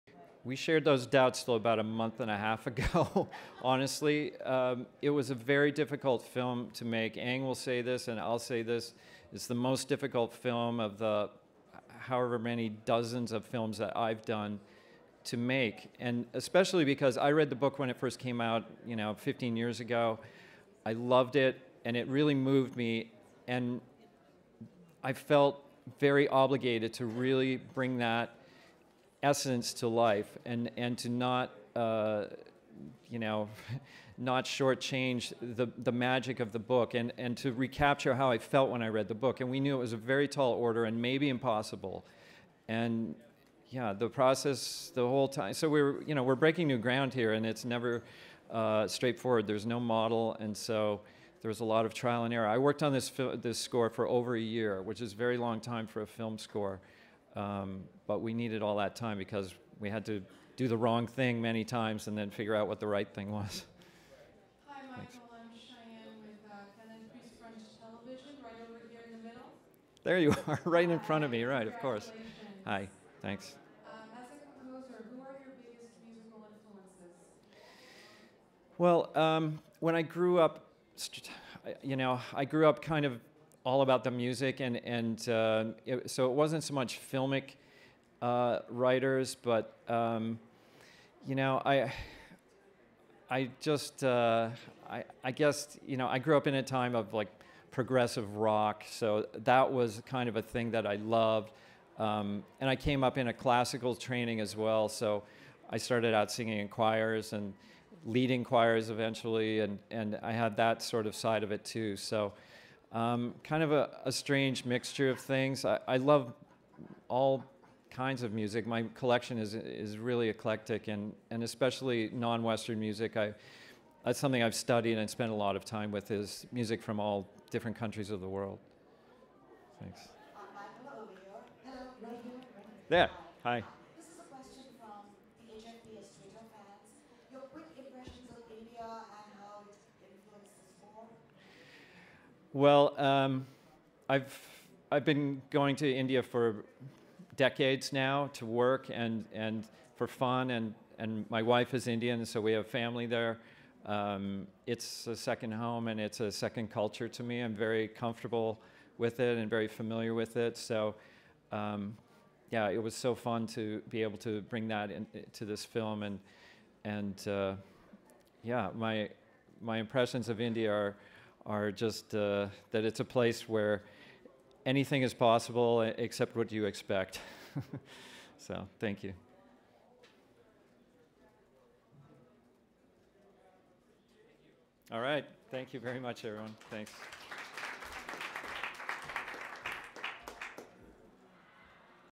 SPEECH - Mychael Danna on sharing doubts with Ang Lee. at...
SPEECH - Mychael Danna on sharing doubts with Ang Lee. at 70th Annual Golden Globe Awards - Press Room 1/13/2013 in Beverly Hills, CA.